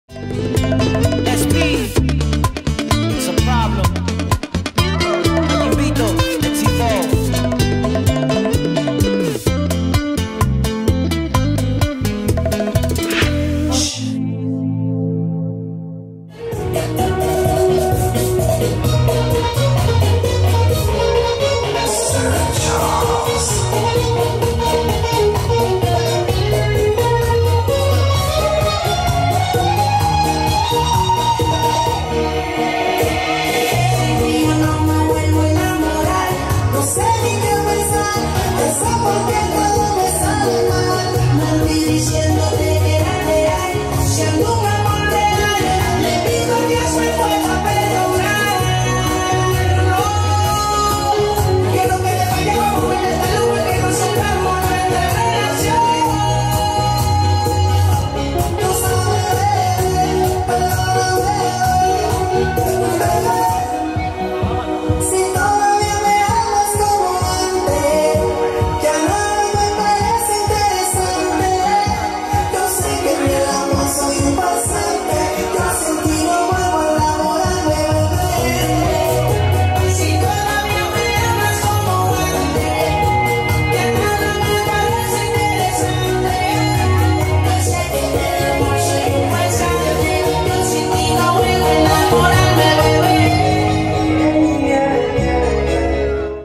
Fabian and Ana Bachata Sensual Social dance| Dance Vida
Bachata and Kizomba Weekend 2019 Stockholm - Sweden
Workshops and party social dance in Salsa Bachata and Kizomba

VIDEO & PHOTOGRAPHY by: Fabian Vallejos - Dance Vida